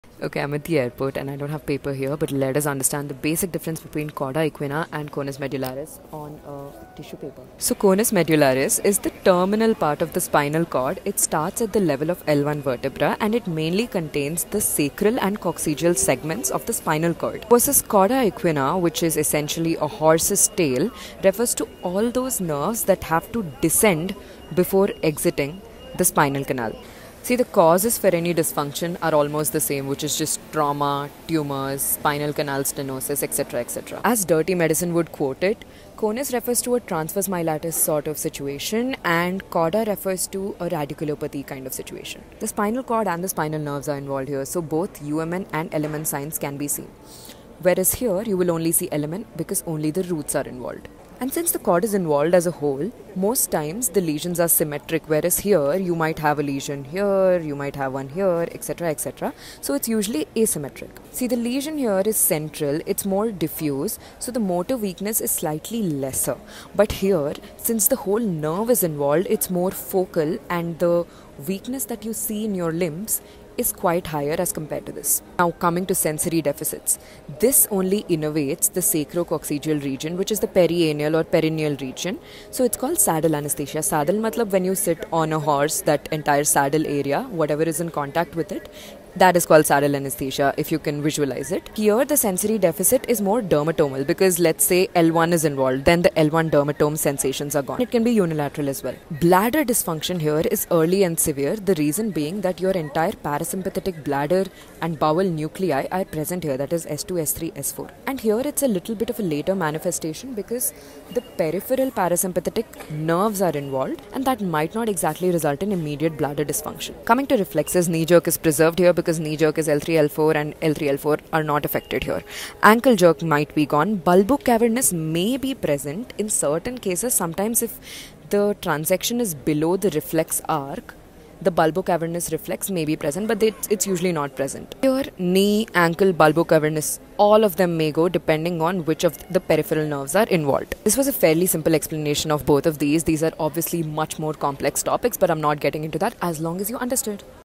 Conus Medullaris vs Cauda Equina, simplified and shortened explanation
missed my flight, so I coped by teaching 😩

conus medullaris, cauda equina, CNS, clinics, medicine, concept, explained,